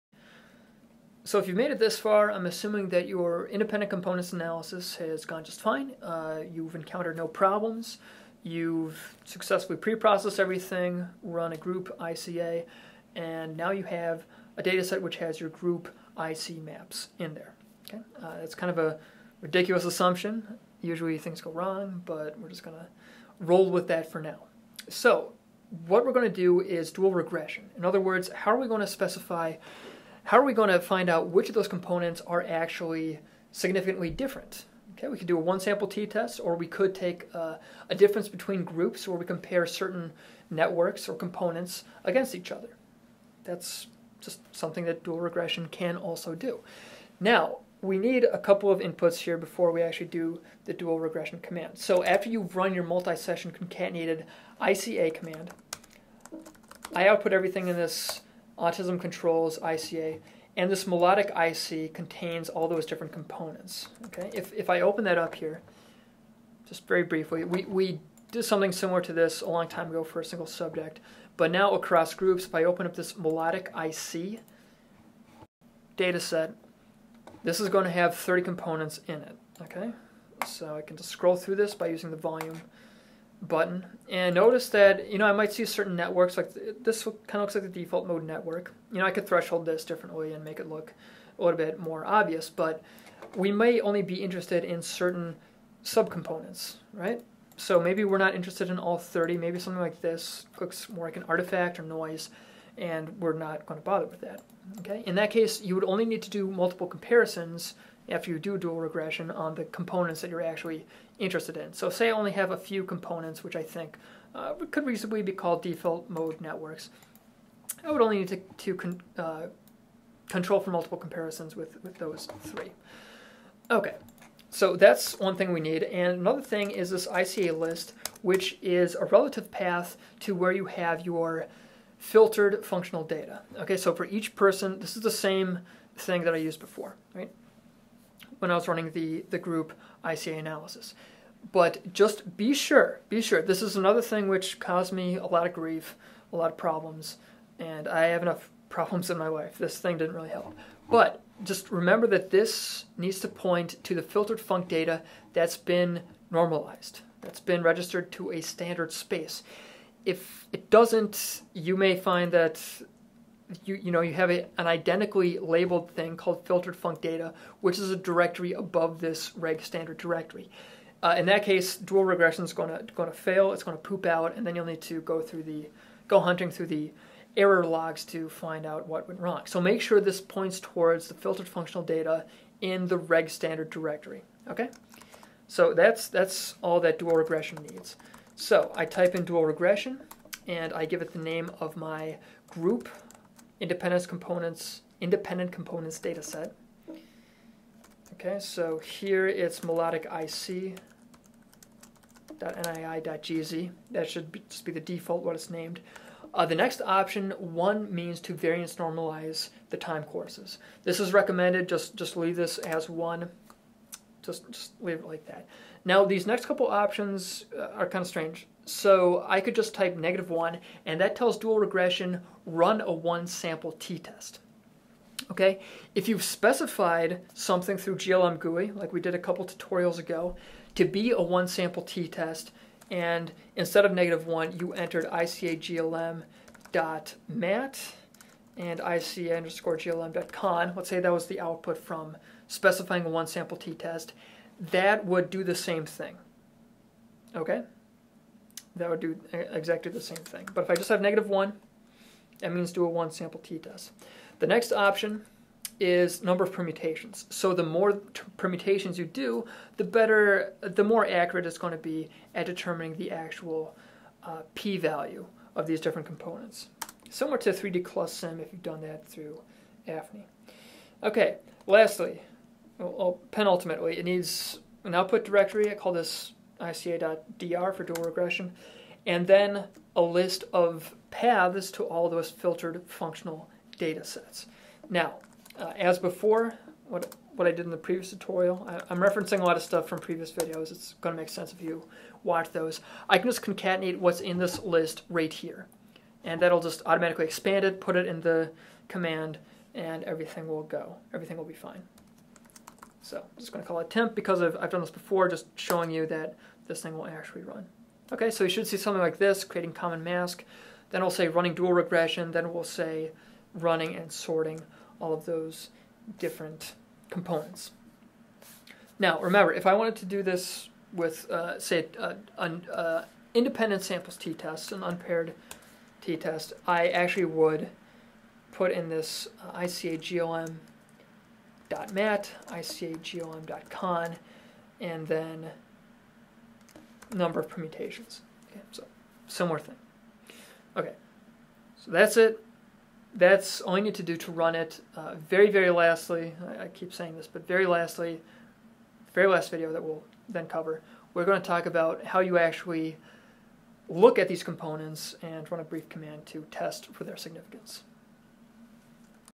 ICA Analysis, Part V: Dual_regression
How to run dual_regression, which outputs corrected components maps.